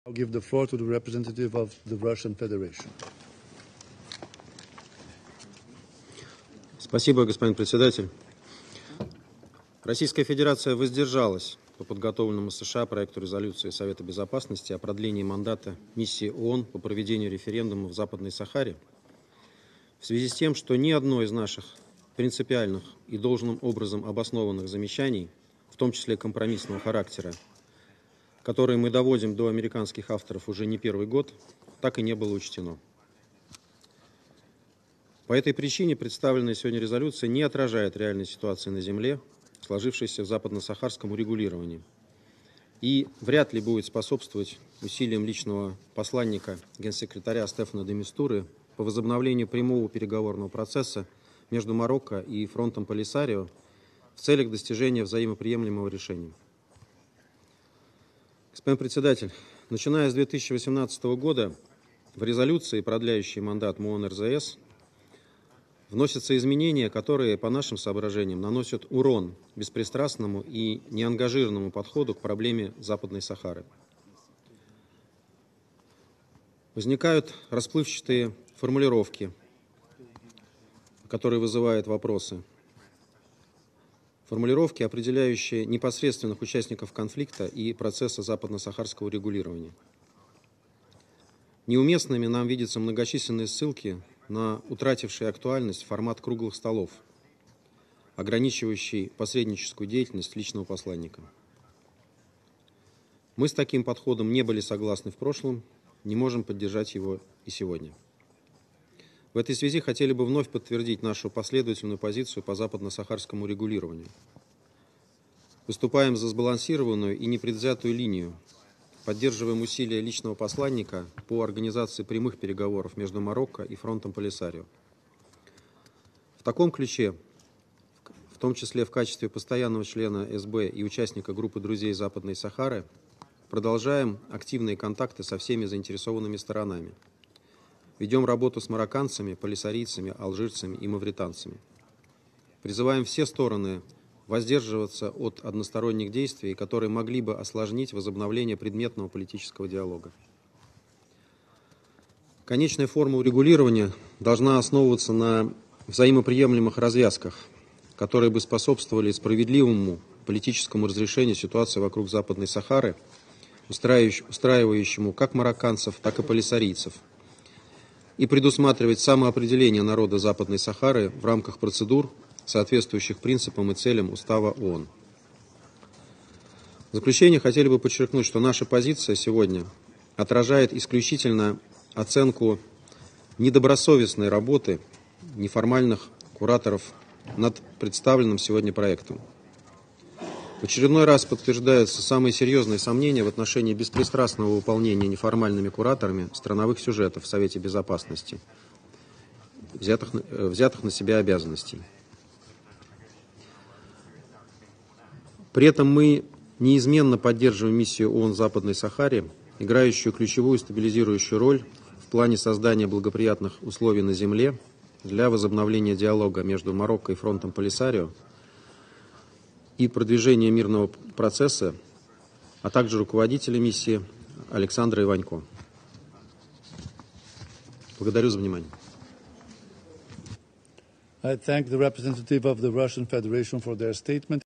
إفادة ممثل روسيا الفدرالية تعليلا للتصويت بالامتناع على مشروع قرار مجلس الأمن بخصوص الصحراء الغربية
إفادة النائب الأول للممثل الدائم ارويكا الفدرالية ديمتري بوليانسكي تعليلا للتصويت على مشروع قرار مجلس الأمن التابع للأمم المتحدة بشأن تمديد ولاية بعثة الأمم المتحدة للاستفتاء في الصحراء الغربية
يجدر الاشارة أن مجلس الامن قد صوت بتمديدة مهمة بعثة المينو سو سنة أخرى بتصويت 13 عضواً بالموتفقة وامتناع روسيا والمؤزمبيق